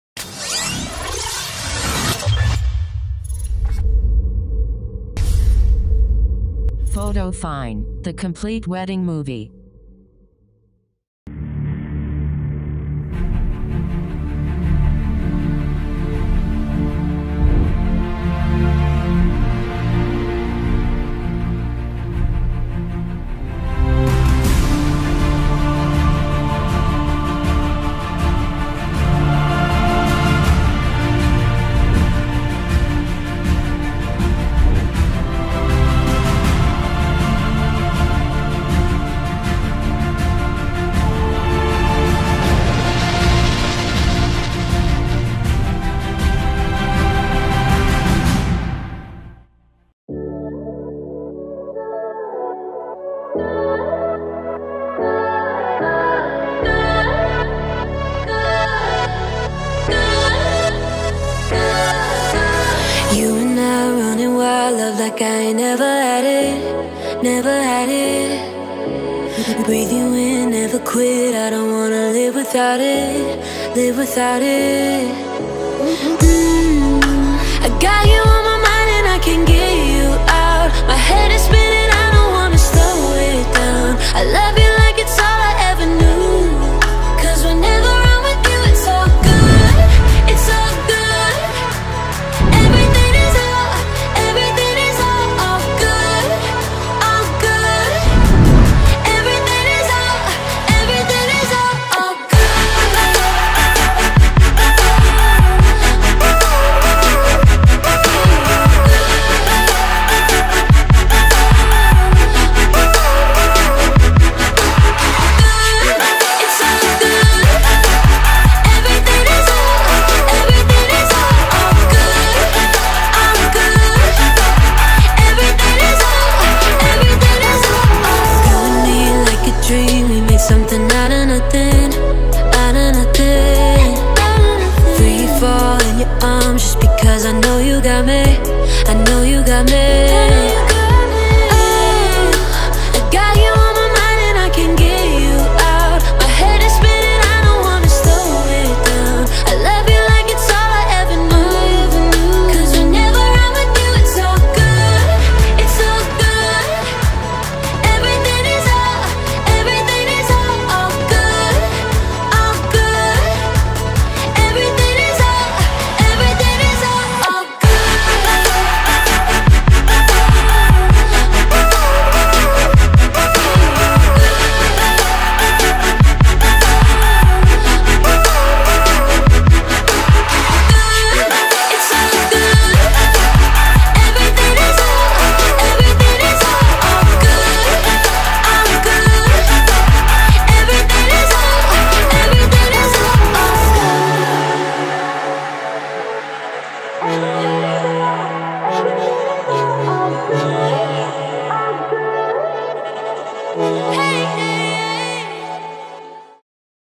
WEDDING HIGHLIGHTS/MUSIC DIRECTOR PRASANTH MOHAN M P & ANCY SAJEEV
THIS IS THE WEDDING HIGHLIGHT VIDEO OF MUSIC DIRECTOR PRASANTH MOHAN MP & ANCY SAJEEV.HE ALREADY DONE MUSIC IN SOOJI.
UPCOMING MOVIES:PATTOM,12:00